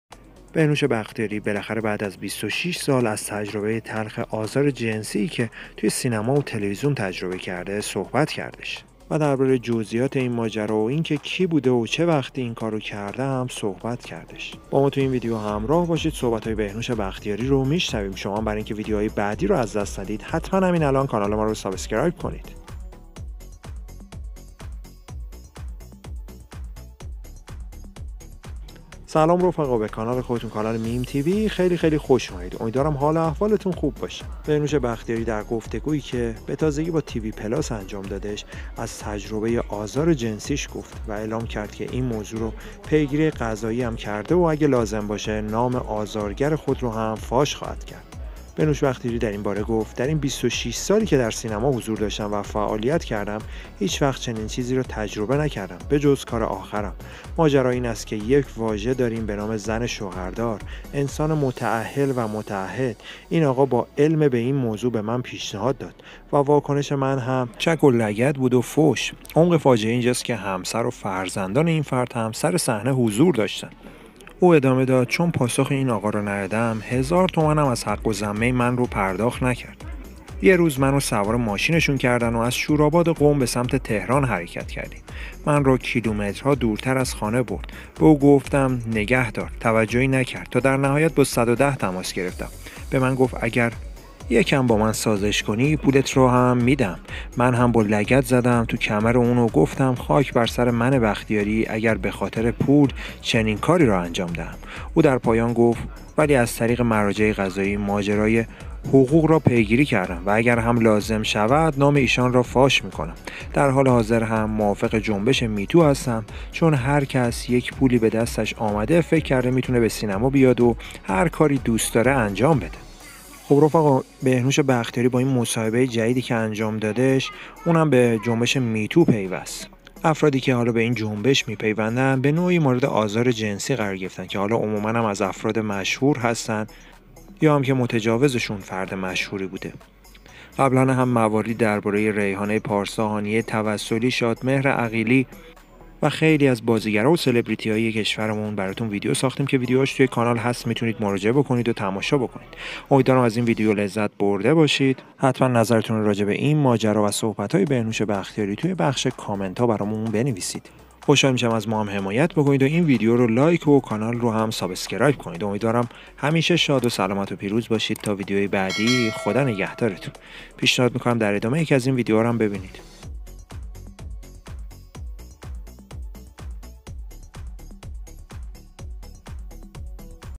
بهنوش بختیاری و افشای تج*اوز و آزار جنس*ی در سینما بعد از 26 سال | افشای جزئیات و متجا*وز
سلام دوستان
 ‌بهنوش بختیاری با روایت تجربه آزار جنس*ی‌ای که دیده است اعلام کرد موافق جنبش می‌تو است و از آن حمایت می‌کند و اگر هم لازم باشد نام آزارگرش را فاش خواهد کرد.

تجربه بهنوش بختیاری از آزار جن*سی
بهنوش بختیاری در گفت‌وگو با یکی از تلویزیون‌های اینترنتی، از تجربه آزار جن*سی گفت و اعلام کرد این موضوع را پیگیری قضایی کرده و اگر لازم باشد نام آزارگر خود را هم فاش خواهد می‌کند.

بهنوش بختیاری در این‌باره گفت: «در این ۲۶ سالی که در سینما حضور داشتم و فعالیت کردم، هیچ‌وقت چنین چیزی را تجربه نکردم به جز کار آخرم. ماجرا این است که یک واژه‌ای داریم به نام زن شوهردار، انسان متاهل و متعد، این آقا با علم به این موضوع به من پیشنهاد داد و واکنش من هم چک و لگد و فحش بود. عمق فاجعه اینجا است که همسر و فرزند این فرد هم سر صحنه حضور داشتند.»

او ادامه داد: «چون پاسخ این آقا را ندادم، هزار تومان از حق‌الزحمه من را پرداخت نکردند، یک روز من را سوار ماشینشان کردند و از شورآباد قم به سمت تهران حرکت کردیم، من را کیلومترها دورتر از خانه برد، به او گفتم نگه دارد توجهی نکرد تا در نهایت با ۱۱۰ تماس گرفتم. به من گفت اگر یکم با من سازش کنی، پولت را هم می‌دهم، من هم با لگد زدم توی کمر او و گفتم خاک‌ بر سر منِ بختیاری اگر به خاطر پول چنین کاری را انجام دهم.»

او در پایان گفت: «ولی از طریق مراجع قضایی ماجرای حقوق را پیگیری کردم و اگر هم لازم شود، نام ایشان را فاش می‌کنم در حال حاضر هم موافق جنبش می‌تو هستم چون هر کسی یک پولی به دستش آمده فکر کرده می‌تواند به سینما بیاید و هرکاری دوست دارد انجام دهد
بهنوش بختیاری و افشای تج*اوز و آزار جنسی در سینما بعد از 26 سال | افشای جزئیات و متجا*وز